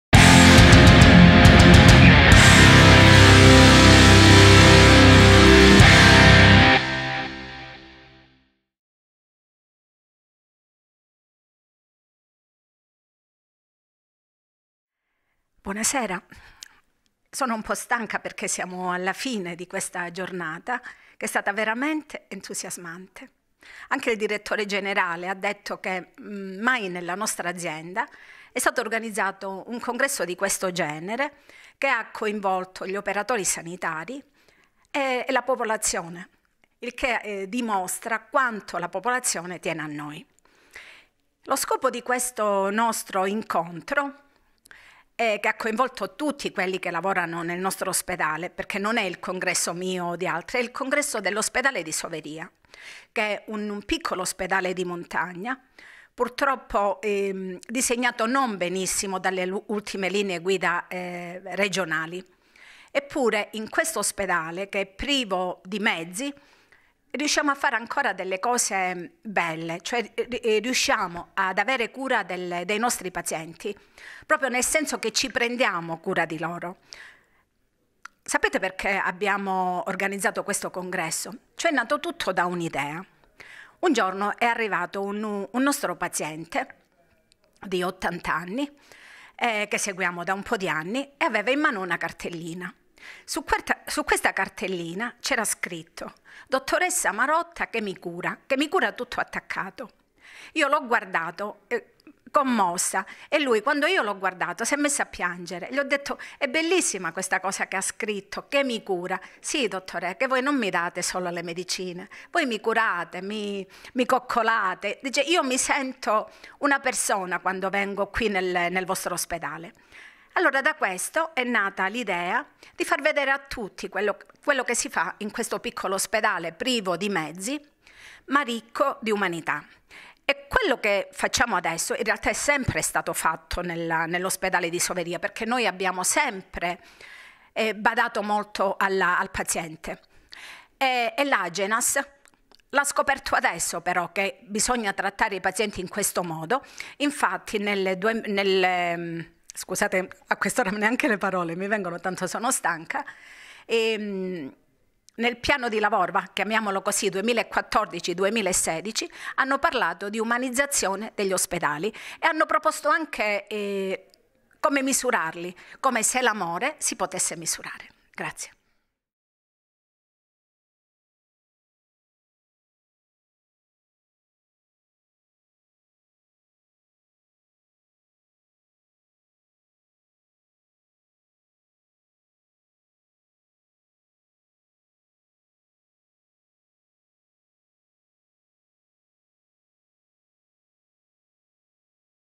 Intervista alla Dott.ssa Anna Marotta - Primario Reparto di Medicina e Cardiologia
“La medicina sobria e rispettosa di un piccolo ospedale di montagna”
Il rilancio del Presidio Ospedaliero di Soveria Mannelli.

11 novembre 2017, presso l'Officina della Cultura e della Creatività di Soveria Mannelli (Cz), organizzato dall’Unità Operativa Complessa di Medicina dell’Ospedale Civile in collaborazione con l’Azienda Sanitaria Provinciale di Catanzaro e con l’Amministrazione Comunale della Città di Soveria Mannelli.